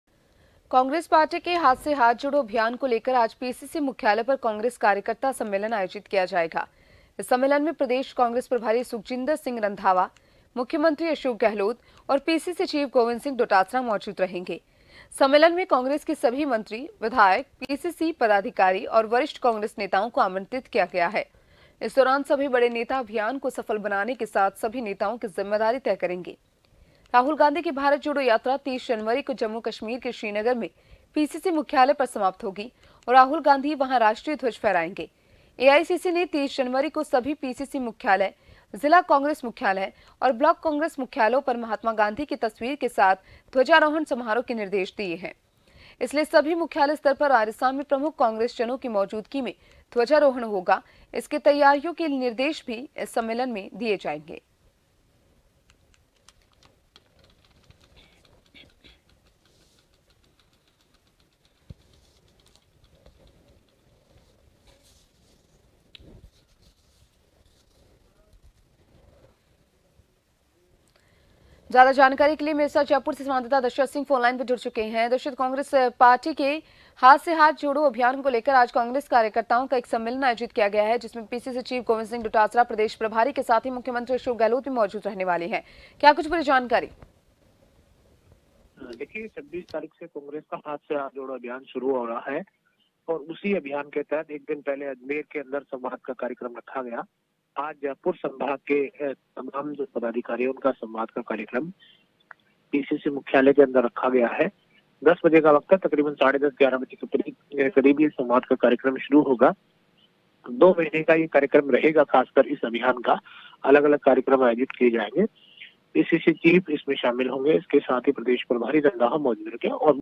Congress का हाथ से हाथ जोड़ो अभियान, आज Jaipur स्थित PCC में होगी बैठक, CM Gehlot आज करेंगे समीक्षा
NEWS CREDIT-GS